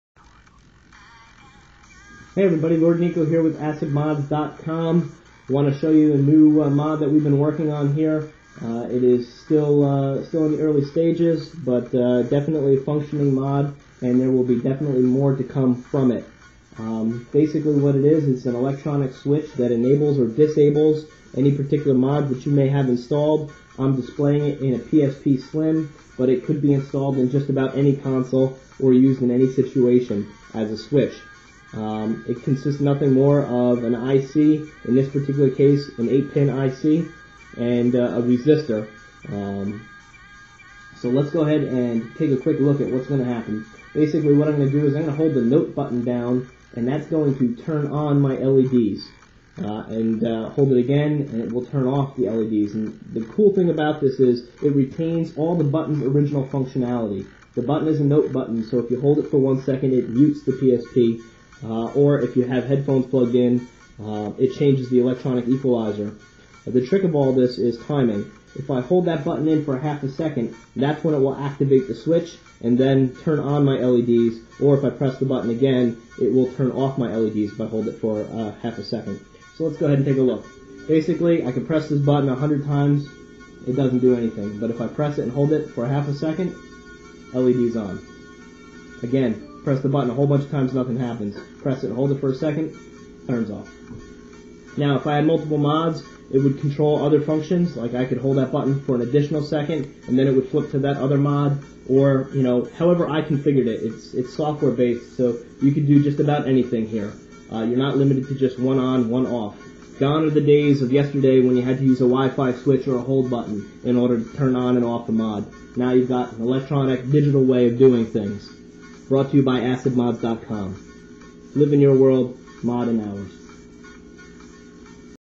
Electronic Switch Mod
Here is an Electronic switch that I have installed in a PSP Slim. This can be installed in any console and be programmed to turn on and of as many mods as you have. The nice thing about this is that it is set to any button you wish and does not interfear with that buttons original functions. To use the button all you need to do is hold it down for .5 of one second.